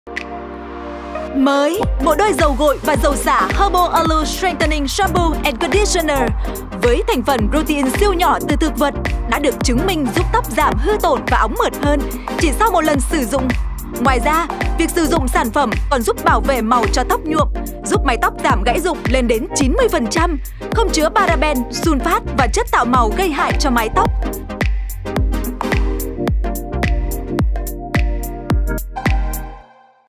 Bộ đôi dầu gội và dầu xả Herbal Aloe Strengthening Shampoo & Conditioner
Bộ đôi dầu gội và dầu xả Herbal Aloe Strengthening Shampoo & Conditioner với thành phần protein siêu nhỏ từ thực vật, được chứng minh, giúp tóc giảm hư tổn và óng mượt hơn chỉ sau một lần sử dụng*

*Khi sử dụng đồng thời Bộ Dầu gội và Dầu xả Herbal Aloe Strengthening Shampoo & Conditioner trên tóc hư tổn đã cho thấy giảm 90% số lượng sợi tóc gãy rụng so với khi không sử dụng trong thử nghiệm Combing